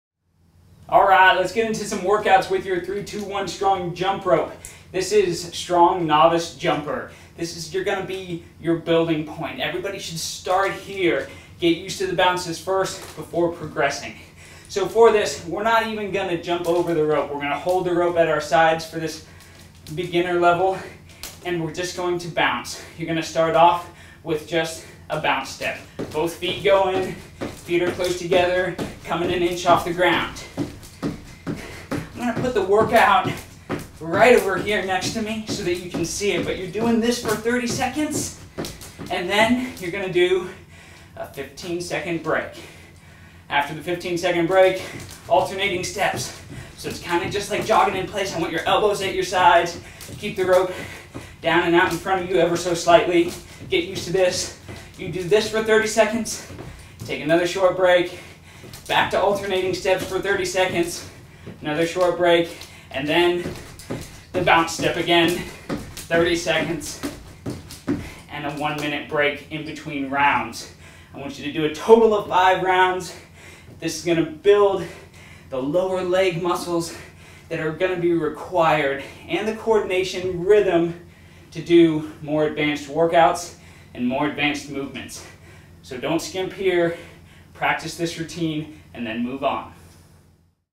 Strong Novice Jumper 321 STRONG Workout Routine
Do 5 sets; increase interval timing and decrease rest periods to increase intensity.
Using the jump rope:
1. Bounce Step for 30 secs; 15 secs rest
2. Alternate Step for 30 secs; 30 secs rest
3. Alternate Step for 30 secs; 30 secs rest
4. Bounce Step for 30 secs; 1 min rest